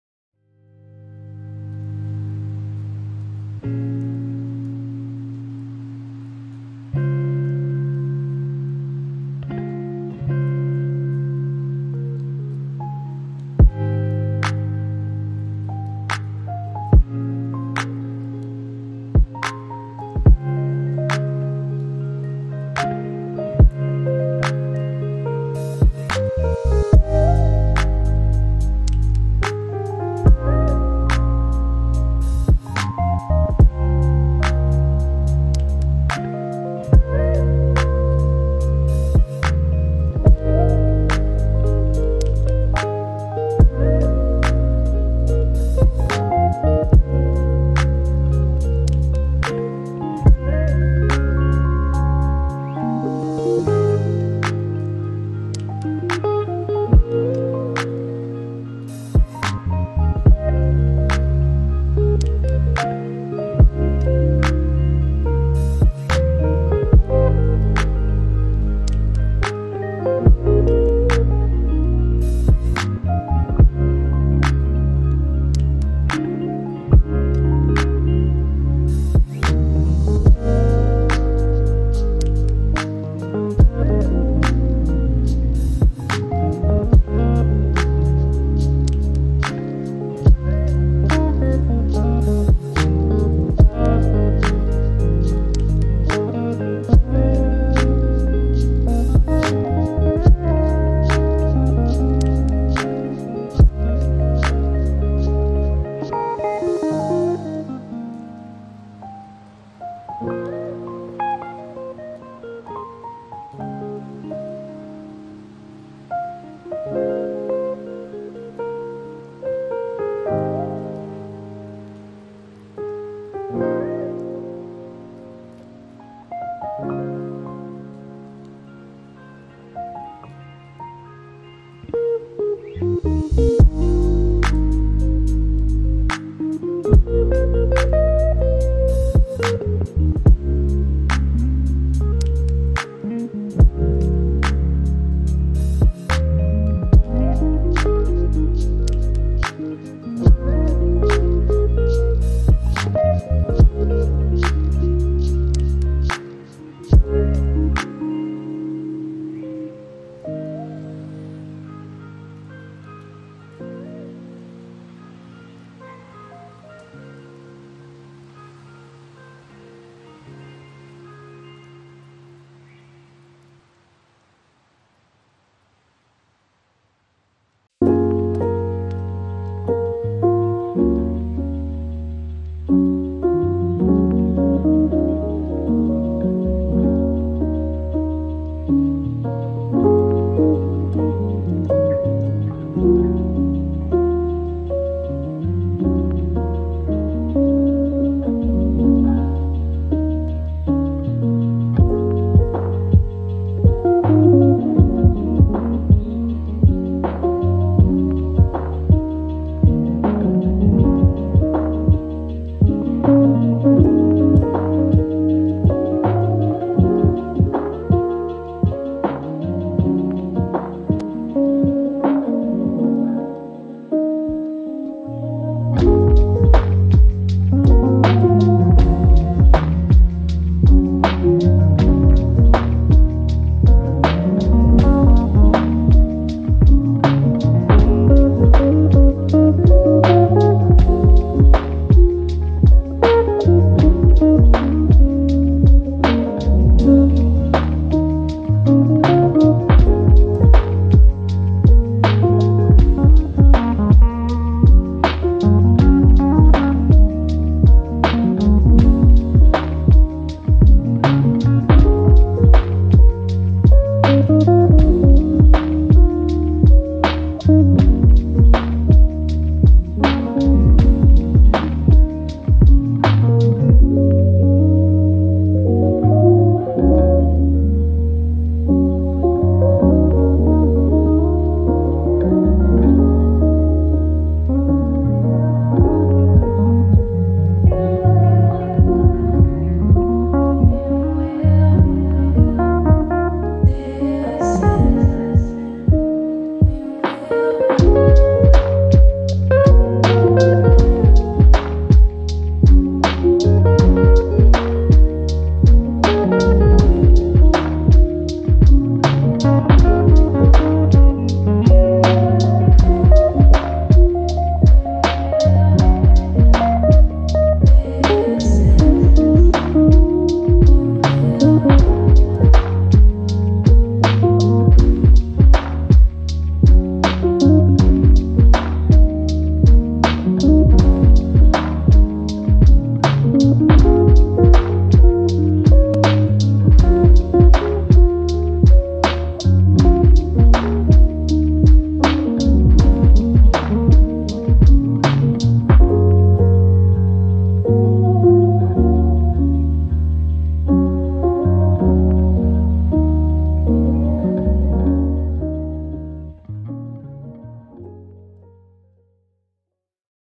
Emoji's Speedpaint #5
This is part 5 of the challenge I set up for myself where I try and turn emojis that are sent to me on TikTok into characters! :D